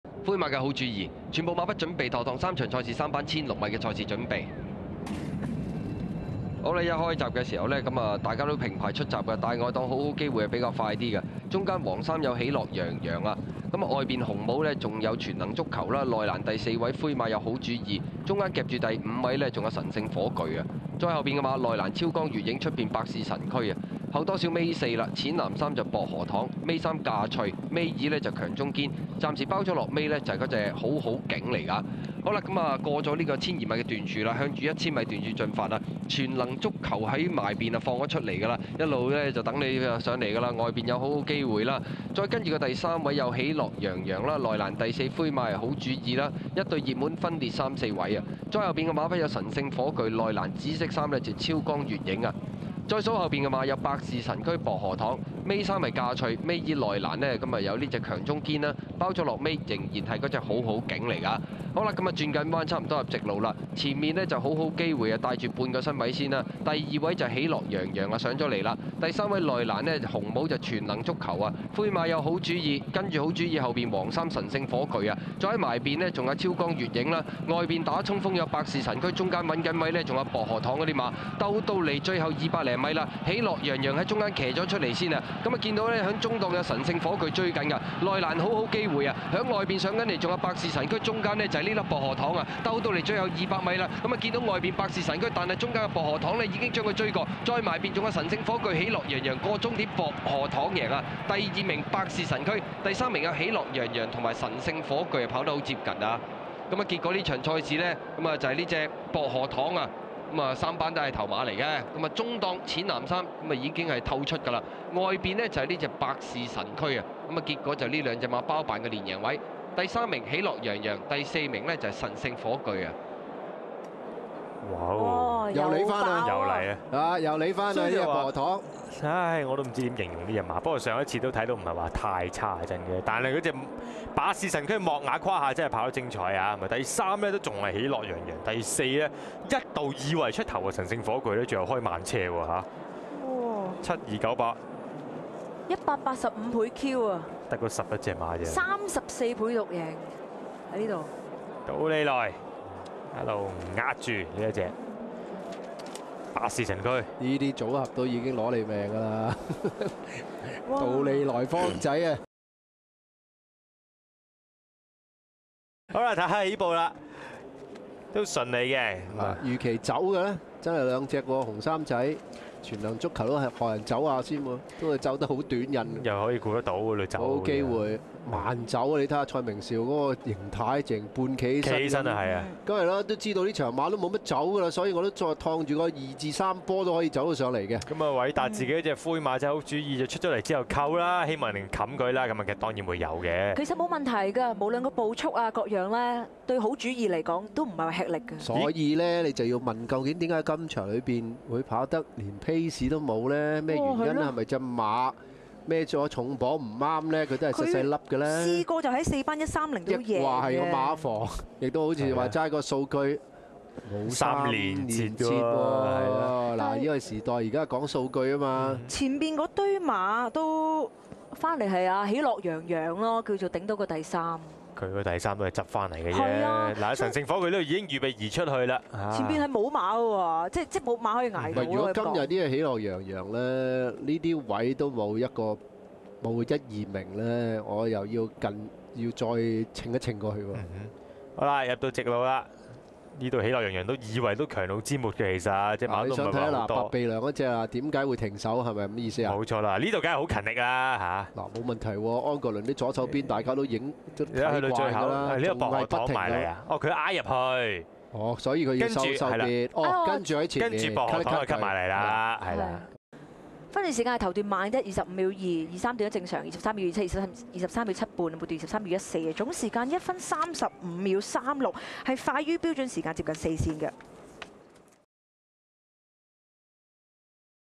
17/03/2013 (463) 薄荷糖 (M192) 杜利萊
17/03/2013 沙田 (場次463)
第三班 - 1600米 - (80-60) 爆冷讓賽
場地狀況 :  好地
賽道 :  草地 - "A"
時間 :  (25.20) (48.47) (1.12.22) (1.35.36)
分段時間 :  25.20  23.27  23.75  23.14